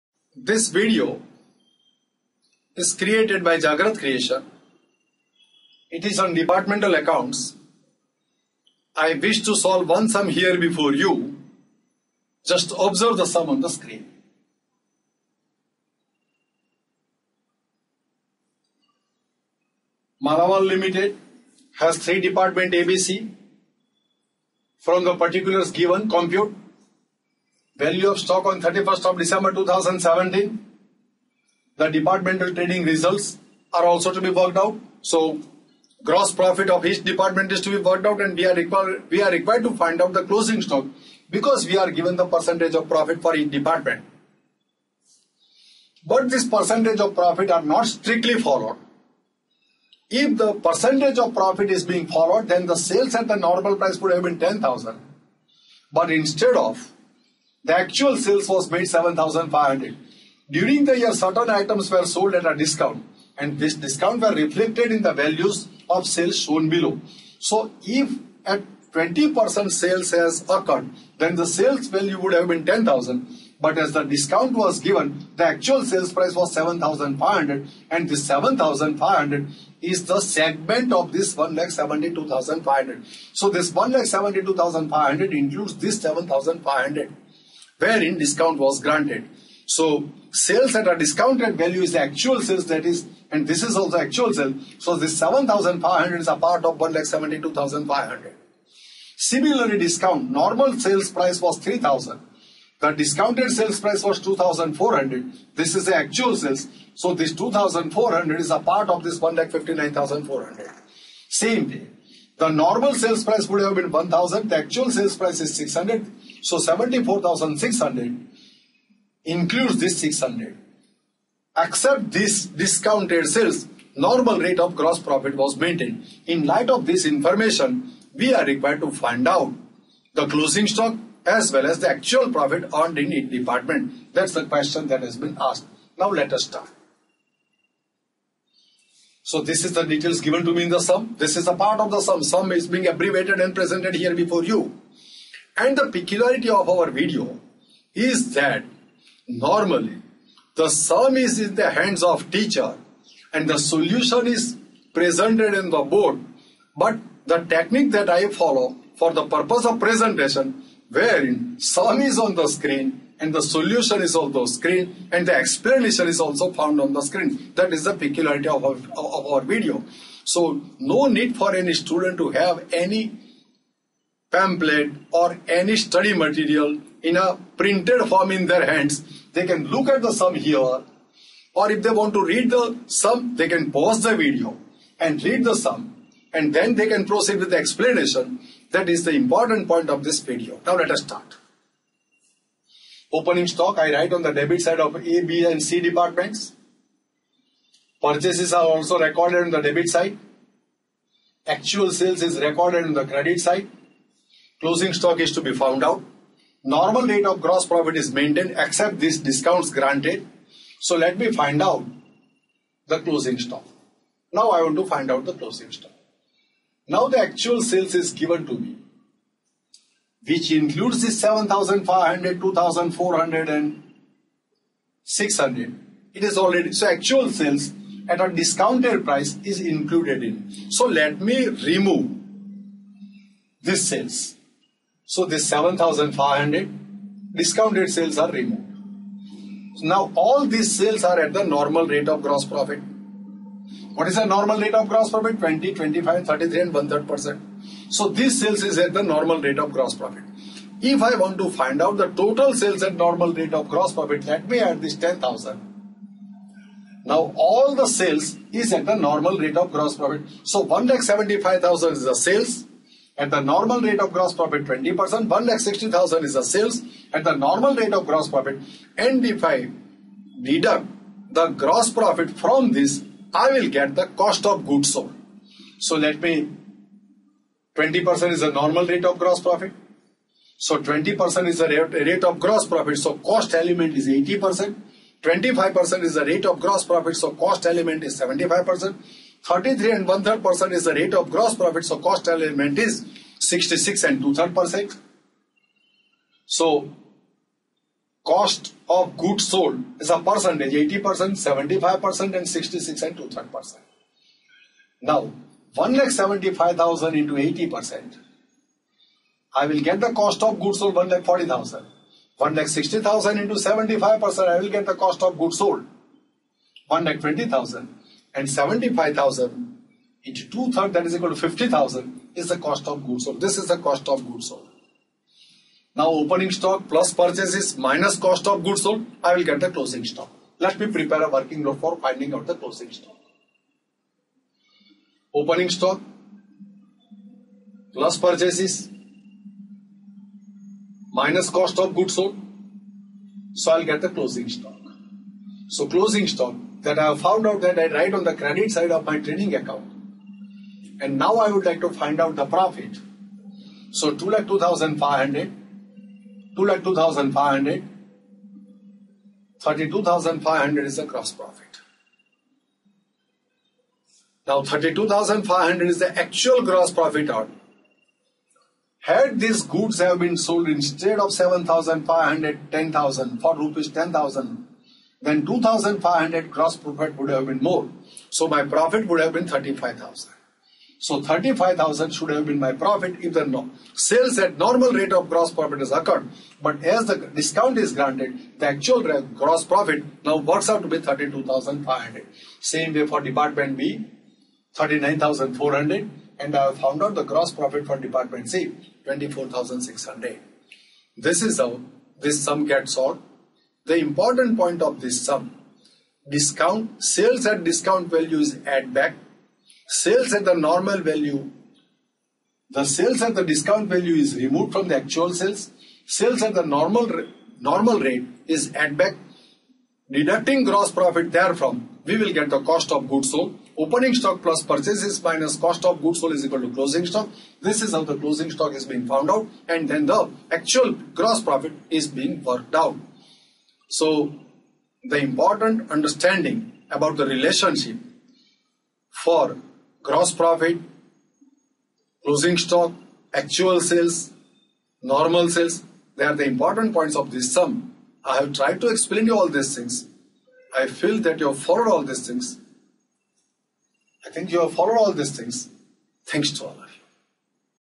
Departmental Accounts Lecture 9
Jagrat Creation Presents This Video lecture on  Departmental Accounts Which Includes sum wherein  Departmental Trading in columnar form, of the three departments is prepared from given information. Important point of Sum is to find closing stock for each Departments.
Keep in touch For Next Accountancy Video on Departmental Accounts.